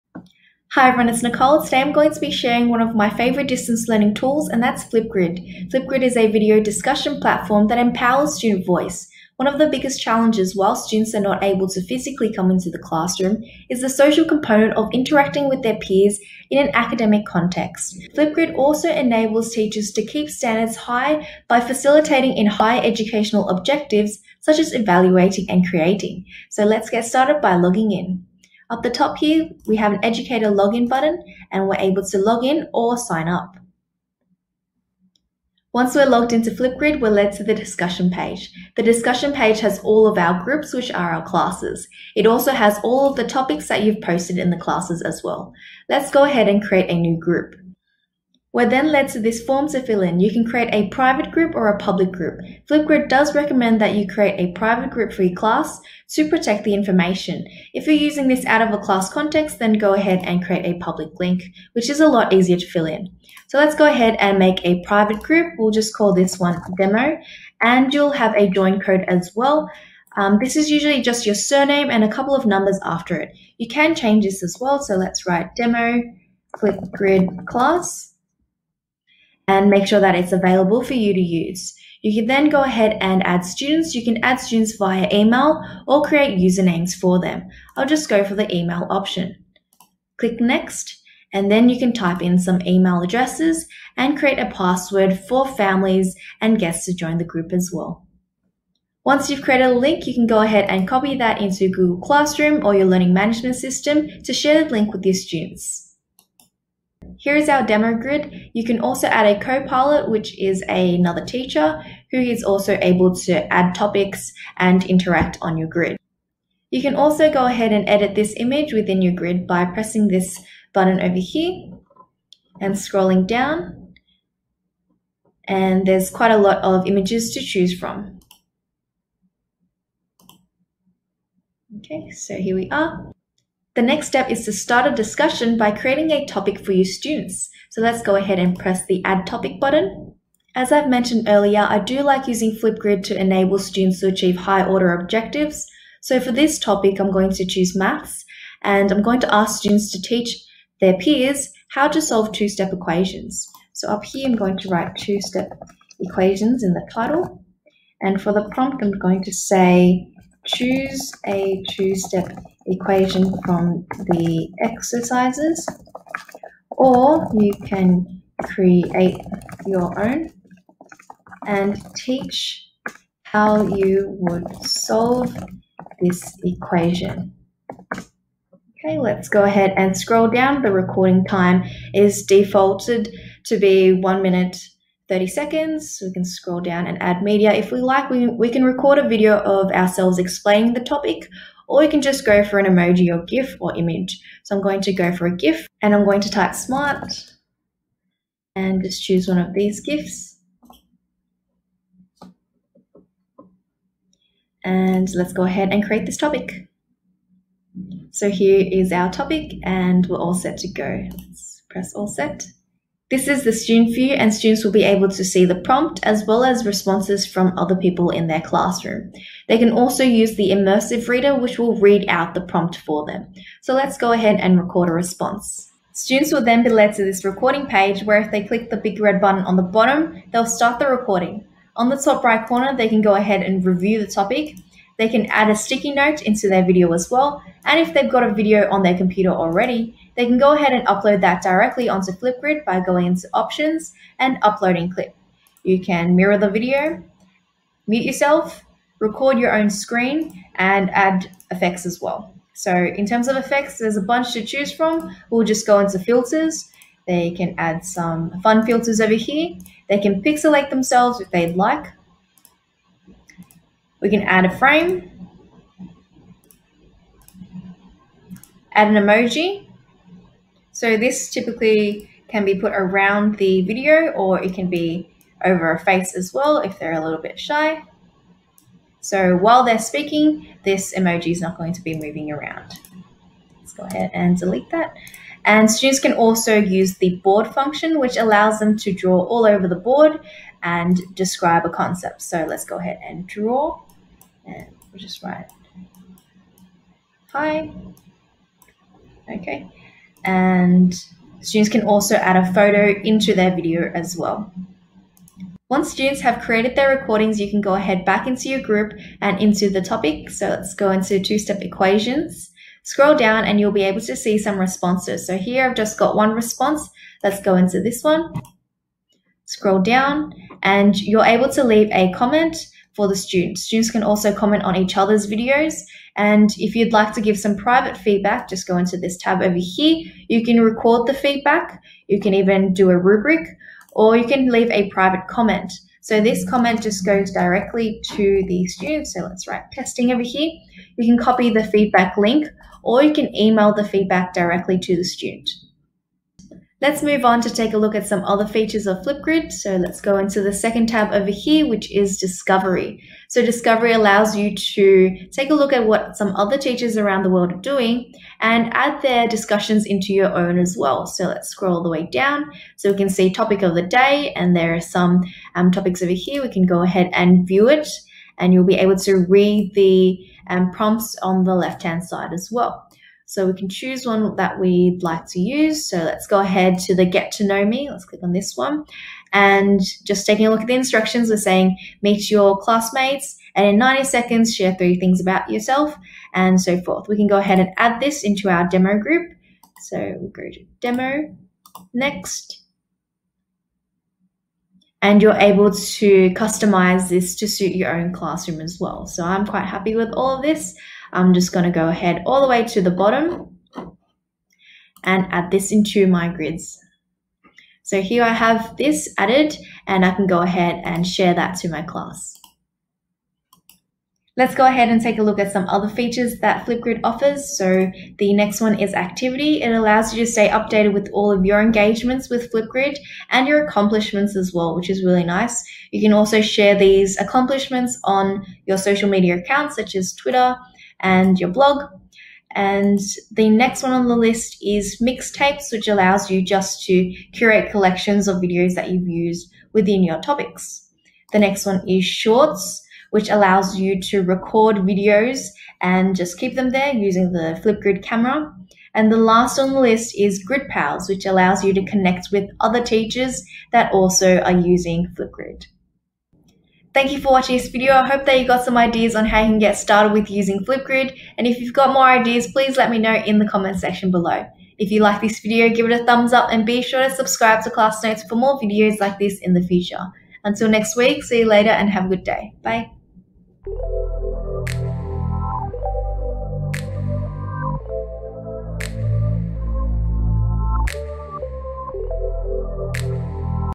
How To Use Flipgrid (The Best Edtech Tool) ✨
Create student-centred lessons by introducing Flipgrid, a video discussion platform into your everyday or online lessons. Teachers create or explore the library of discussion prompts and assign it for students. You can be as creative as you want! Students will then record a video response to contribute their thoughts and idea to the class. They can view and engage with responses by their peers! Share your grids as a link to any platform that you use to get students started!

About
Hi friends, I'm Nicole! 👋 I'm an educator who shares beginner friendly teaching + tech ideas to help you have fun and create engaging experiences for your class.

Timestamps
0:00 Intro
0:35 Discussion Tab
0:46 Create a Group
1:46 Group Page
2:18 Create a Topic
3:33 Student View of the Grid
3:44 Record a Video Response
5:05 Commenting & Providing Private Feedback
5:52 Discovery Tab (Resource Library)
6:54 Other Features
7:36 Thank you for watching!